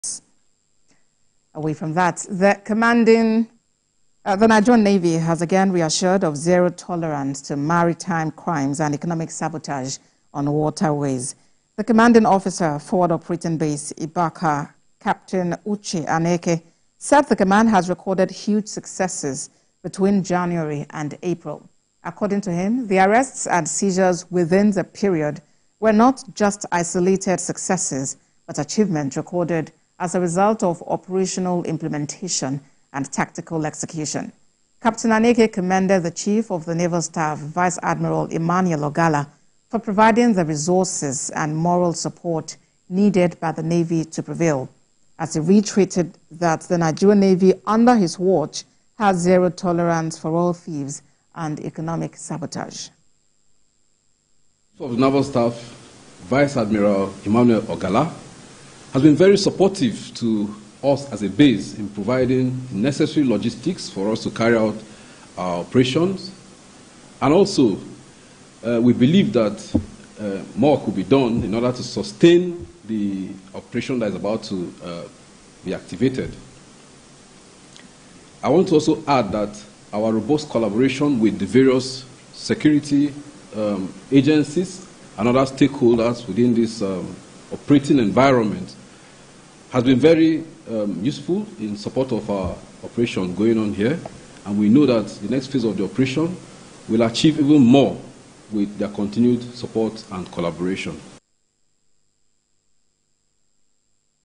Navy Reiterates Commitment To Protecting Nigeria's Waterways
The Nigerian Navy has again reassured of zero tolerance to Maritime crimes and Economic sabotage on waterways.
The Commanding Officer, Forward Operating Base, Ibaka, Capt. Uche Aneke said the command has recorded huge successes between January and April.
According to him, the arrests and seizures within the period were not just isolated successes but achievements recorded as a result of operational implementation and tactical execution.
Captain Aneke commended the Chief of Naval Staff, Vice Admiral Emmanuel Ogalla, for providing the resources and moral support needed by the Navy to prevail,  as He reiterate that the Nigerian Navy under his watch has zero tolerance for oil thieves and economic sabotage.